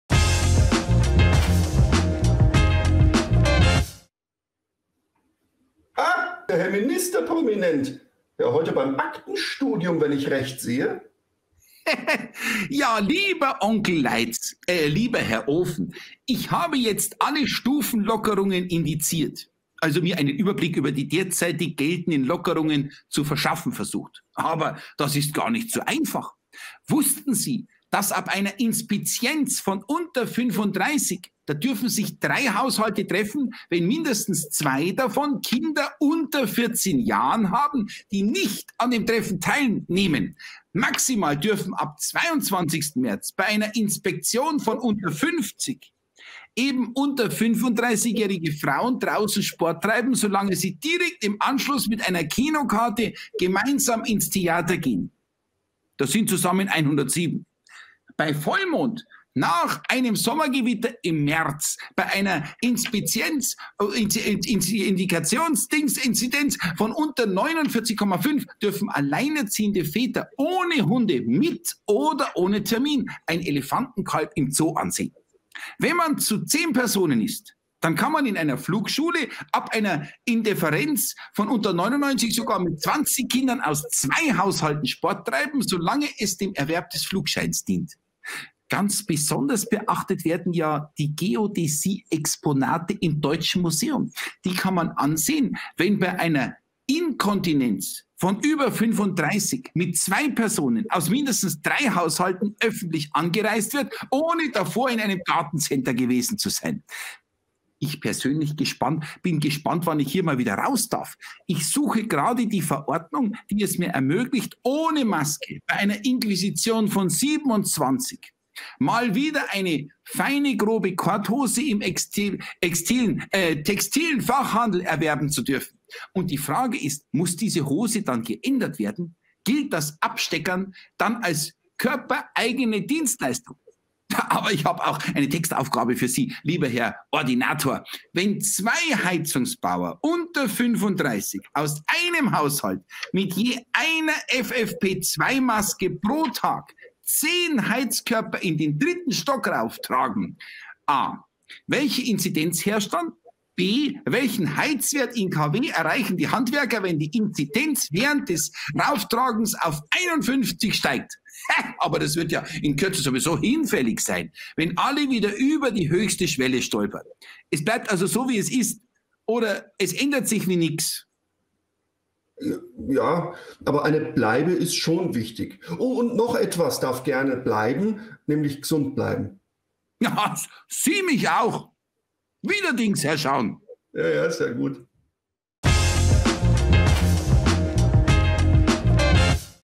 Der Ministerprominent als Inzidenz-Kompetenz
Der Dings aus Hausratswolfen hat einfach den Durchblick und bringt für uns Licht ins Dunkel des Inzidenz-Irrsinns!